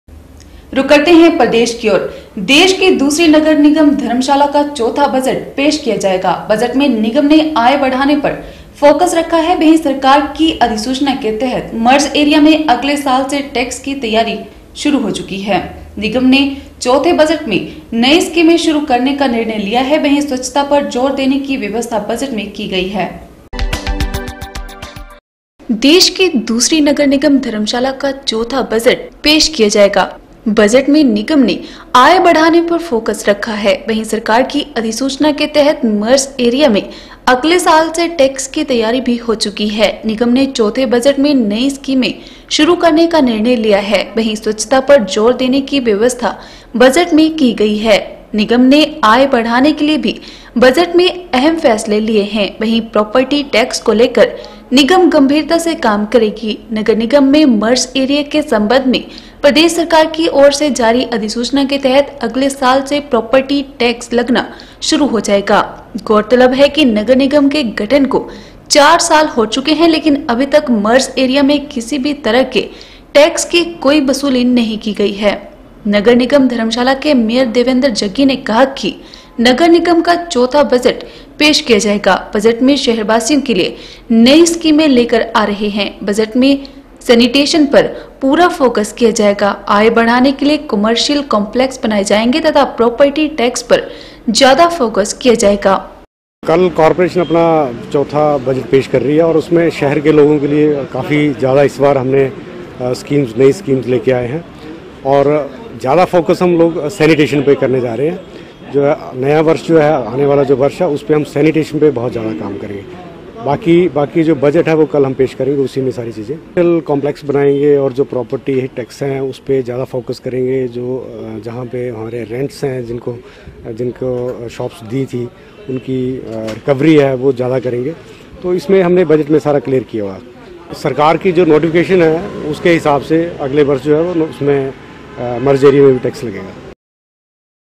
25 FEB N 7 आय बढ़ाने पर फोकस करेगी नगर निगम
देश की दूसरी नगर निगम धर्मशाला का चौथा बजट मंगलवार को पेश किया जाएगा। बजट में निगम ने आय बढ़ाने पर फोकस रखा है, वहीं सरकार की अधिसूचना के तहत मर्ज एरिया में अगले साल से टैक्स की तैयारी भी हो चुकी है। निगम ने चौथे बजट में नई स्कीमें शुरू करने का निर्णय लिया है, वहीं स्वच्छता पर जोर देने की व्यवस्था बजट में की गई है।